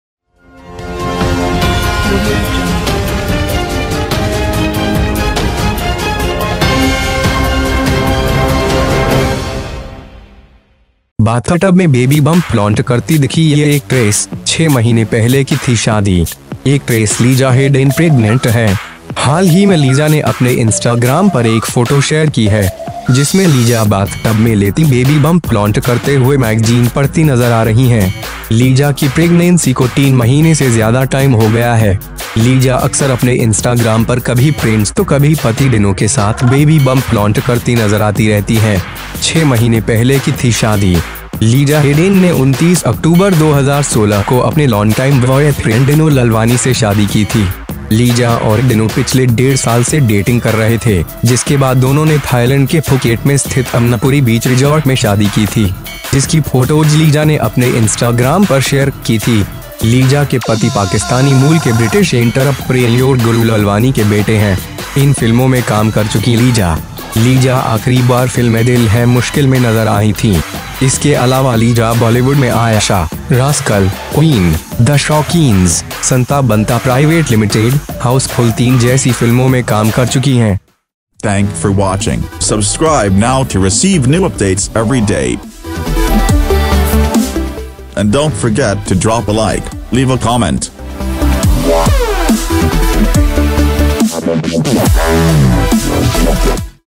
बाथटब में बेबी बंप फ्लॉन्ट करती दिखी ये एक्ट्रेस, 6 महीने पहले की थी शादी | 2 Minute News Today
बाथटब में बेबी बंप फ्लॉन्ट करती दिखी ये एक्ट्रेस, 6 महीने पहले की थी शादीएक्ट्रेस लीजा हेडन प्रेग्नेंट हैं। हाल ही में लीजा ने अपने इंस्टाग्राम पर एक फोटो शेयर की है। जिसमें लीजा बाथ टब में लेतीं बेबी बंप फ्लॉन्ट...Bollywood actress Lisa Haydon shares her baby bump photo
Actress Lisa Hayden is pregnant Lisa recently shared a photo share on her Instagram. In which Lisa is taking a bath in a bathtub and doing a baby bump and reading the magazines. Liza's pregnancy has been more than 3 months. Lisa has often seen friends on her Instagram as a baby bump with her husband and sometimes with a Dino. 6 months ago was married ...
Lisa Hayden married her long time boyfriend Dino Lallawani on October 29, 2016. Liza and Dino were dating for the last one and a half years, after which they were married at the Amanpuri Beach Resort located in Phuket, Thailand. Whose pictures Lisa shared on her Instagram. Lisa's husband is the son of British entrepreneur Gulu Lalwani of Pakistani origin.
Lisa, who worked in these films
Liza was last seen in the film 'A Dil Hai Katha'. Apart from this, Lisa has worked in films like 'Aisha', 'Rascal', 'Queen', 'The Shockings', 'Santa Banta Private Limited', 'Housefull 3'.